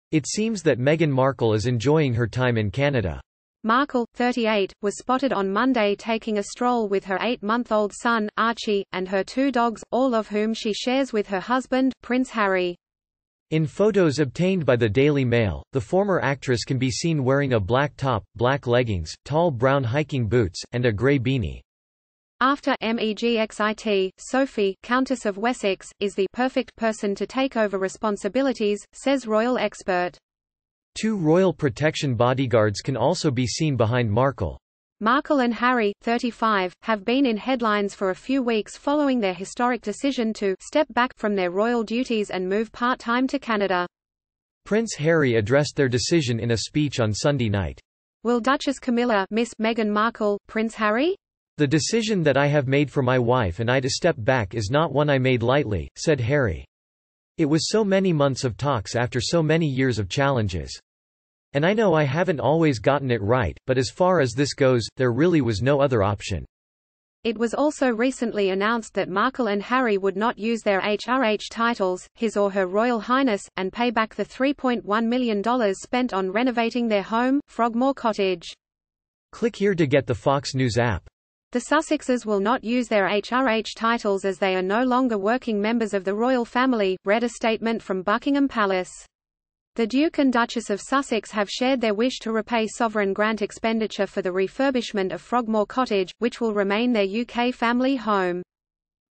Meghan Markle steps out with baby Archie after 'Megxit' announcements
It seems that  Meghan Markle  is enjoying her time in Canada.